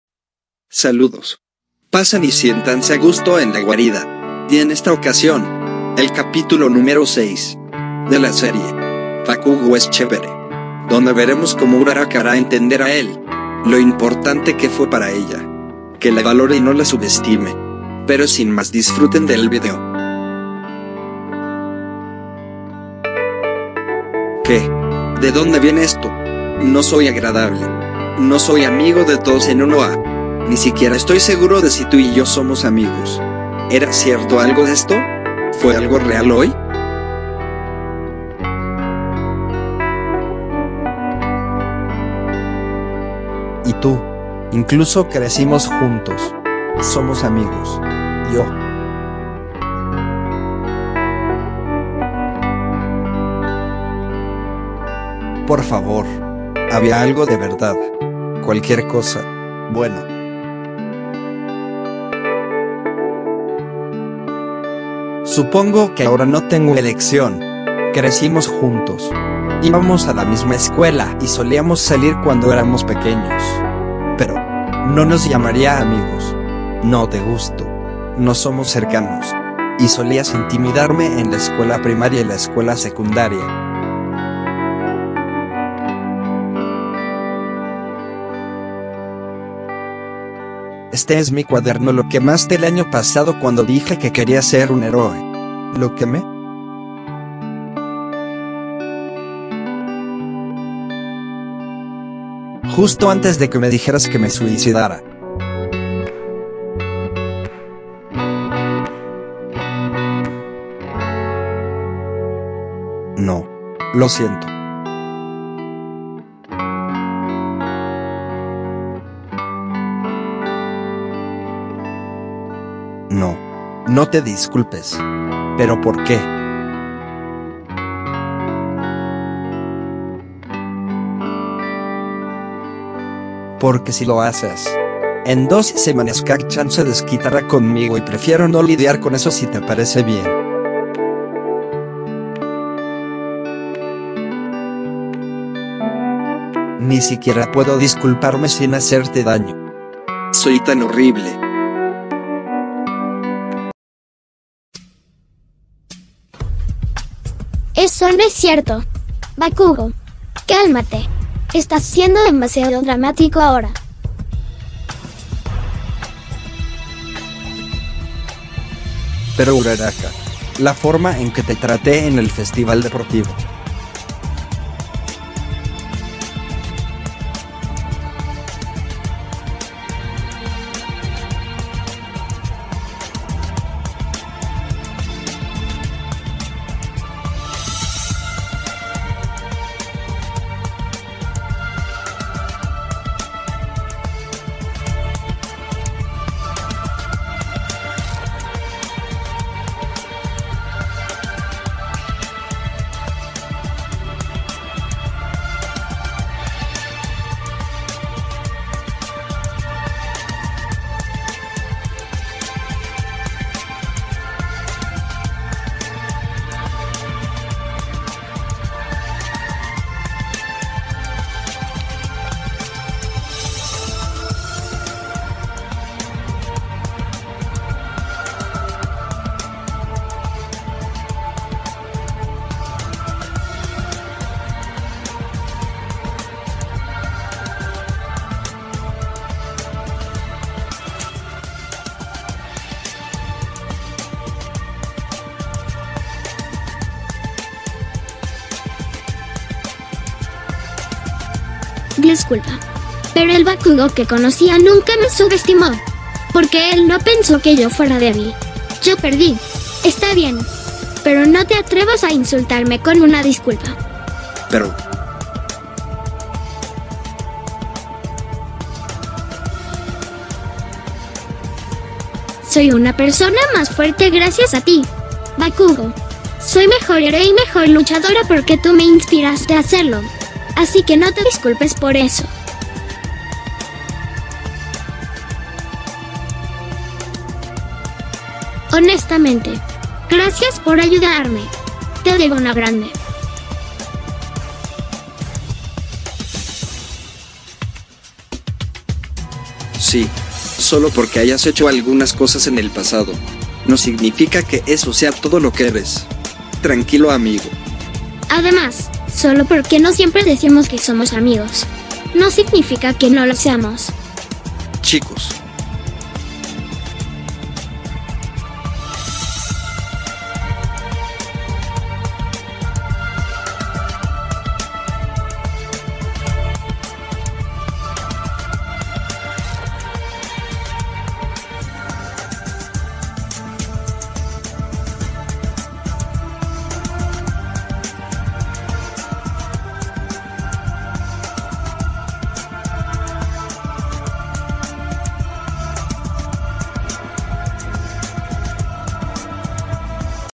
Bakugou es chévere - Kacchako - Capítulo 6 - Uraraka agradece a Bakugou por reconocer su fuerza
Y tras haberse enterado la verdad por parte de Mineta, el nuevo Bakugou en un mar de lagrimas, le reclamará a todos por haberle mentido, será en ese momento que Uraraka le aclarará las cosas.